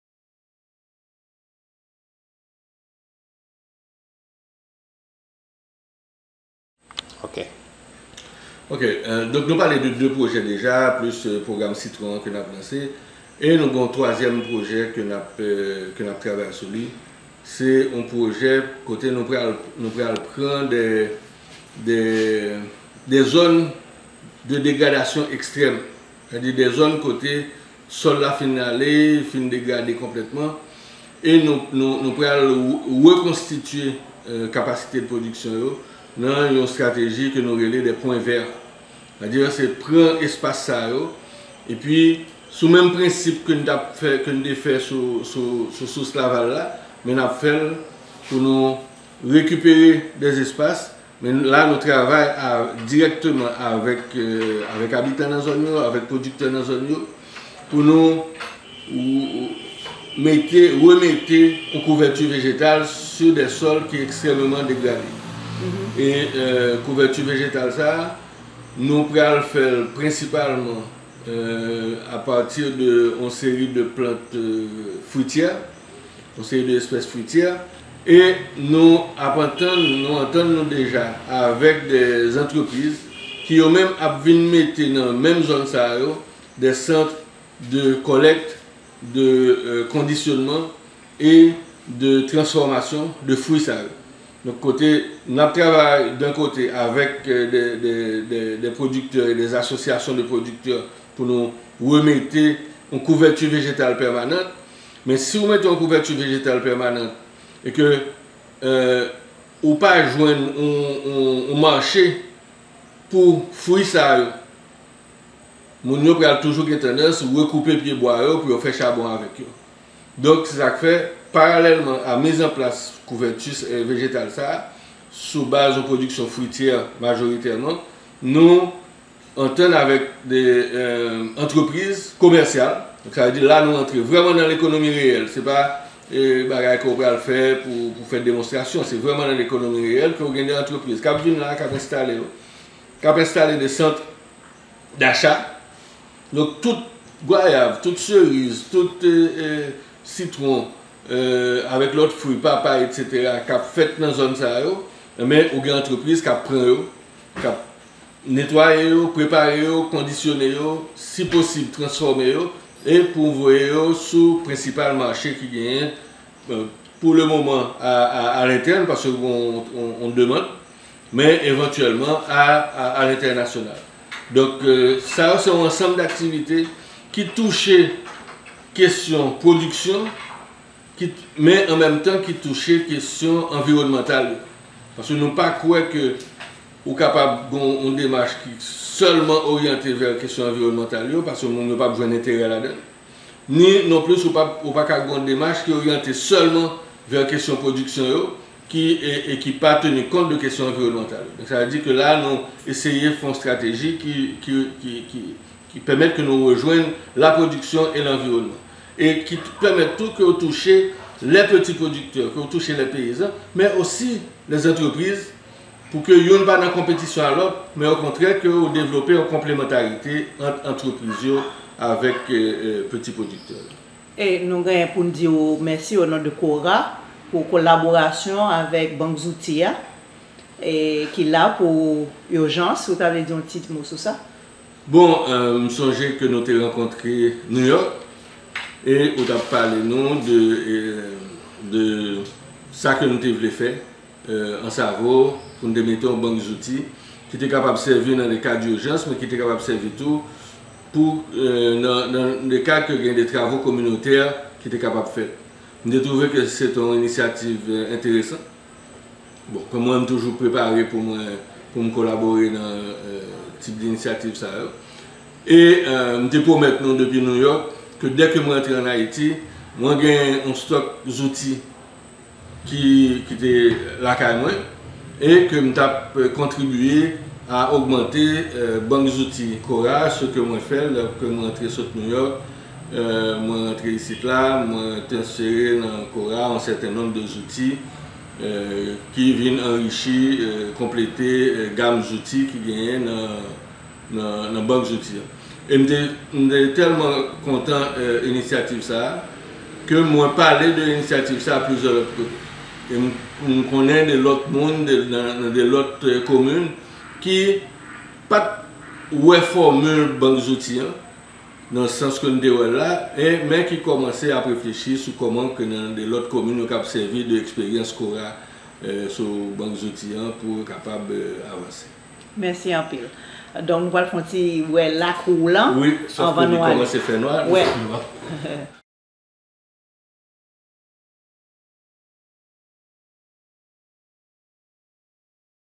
Carl Monde-Project for Anse-a-Veau, Nippes, Haiti
Part of Carl Monde's interview about his life in Anse-a-Veau and the projects he is trying to realize for the development of the town and the Nippes.  He also talks about the contribution made to CORA's Depozouti (tools bank) for community work and emergency.
Mr. Monde is a great supporter to make changes in the town.